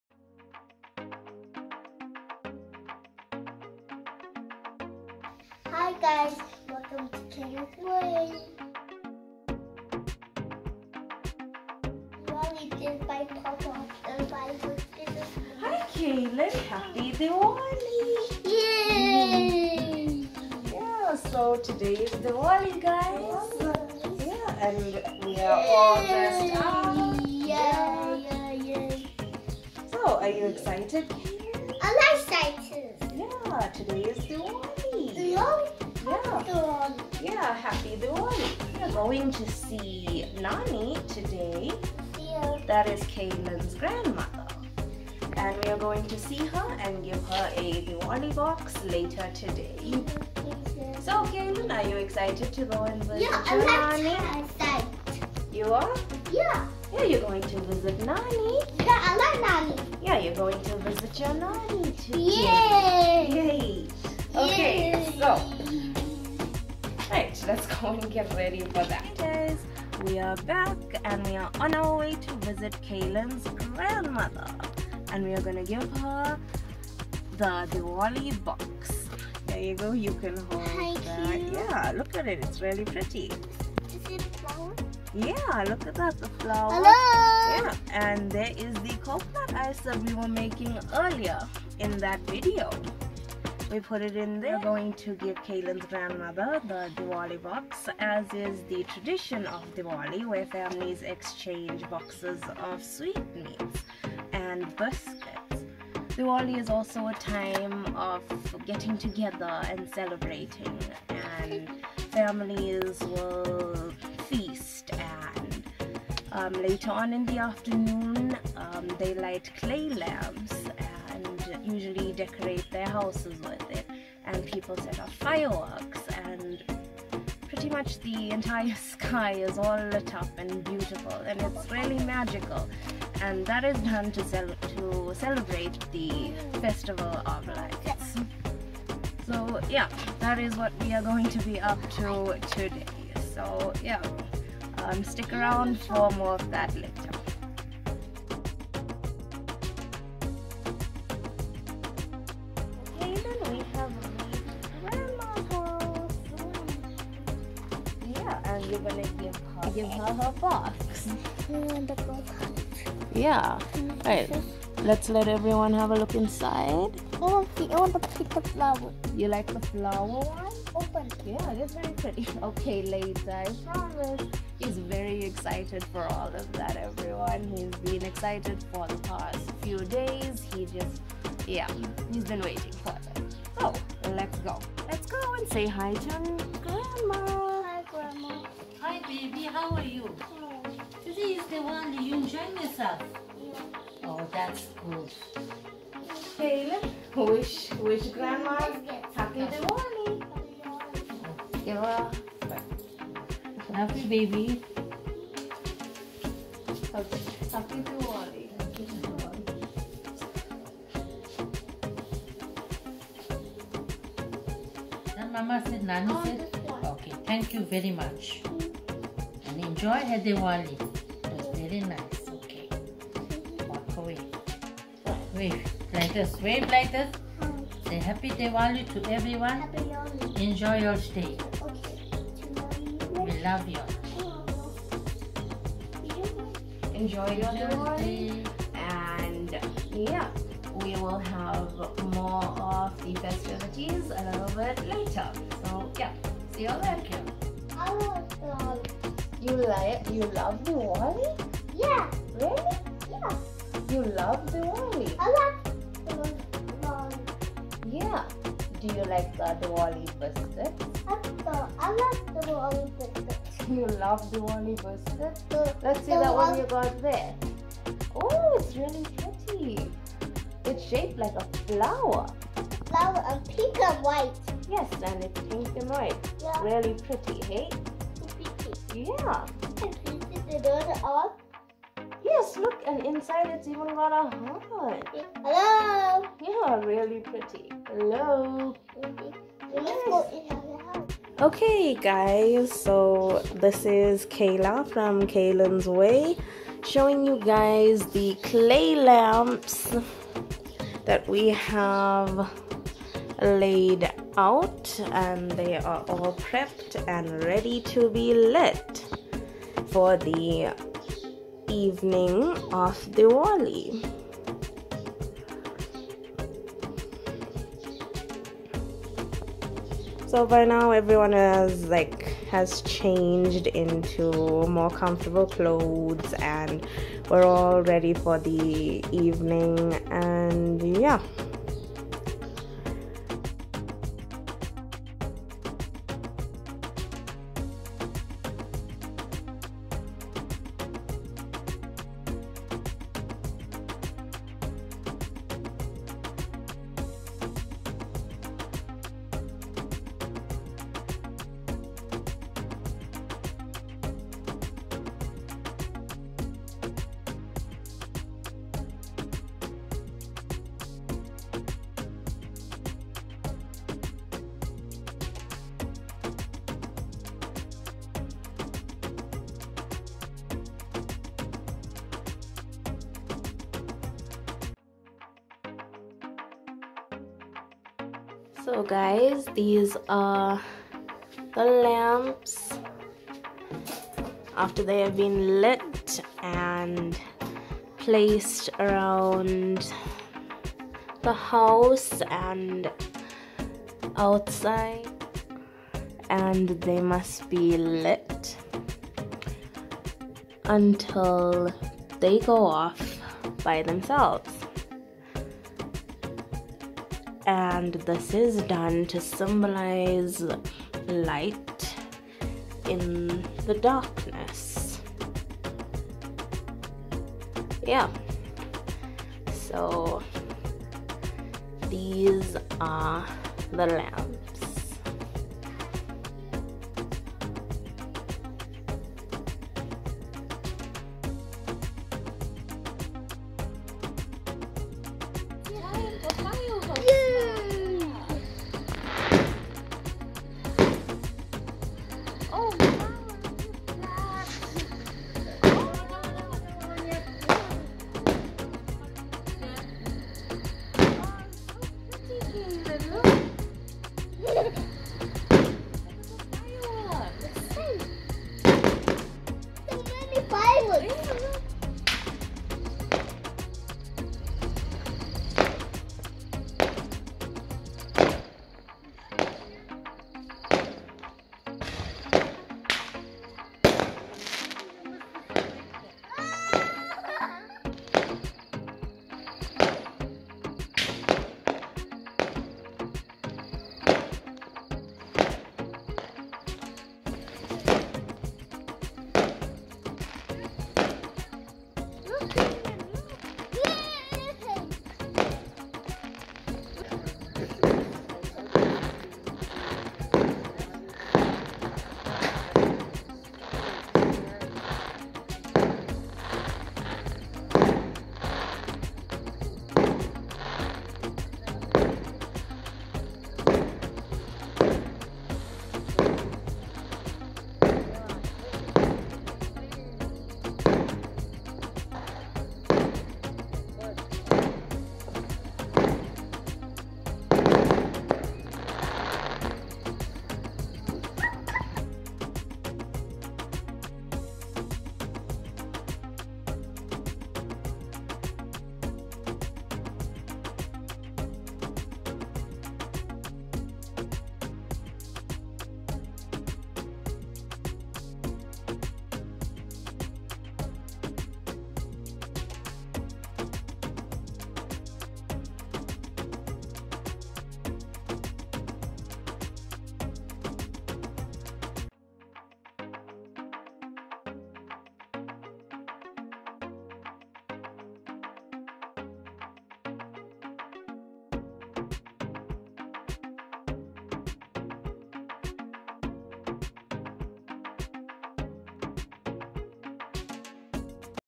Kaylen's Diwali (Diwali part 2)
Kaylen celebrates Diwali with his family